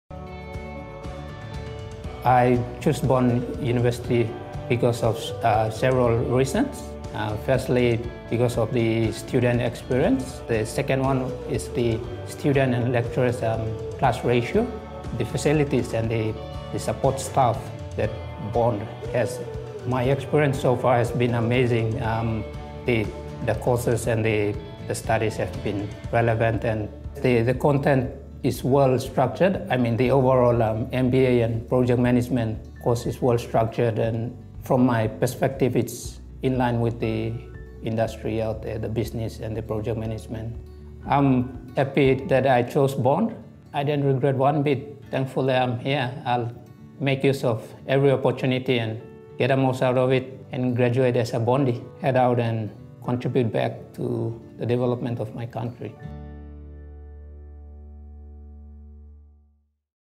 I chose Bond because of the student experience
Emmanuel David from Papua New Guinea (PNG) chose to study his combined MBA and Project Management degree at Bond University in Australia for many reasons, including the low staff to student ratio, the great facilities and support staff, and the fantastic student experience offered on campus.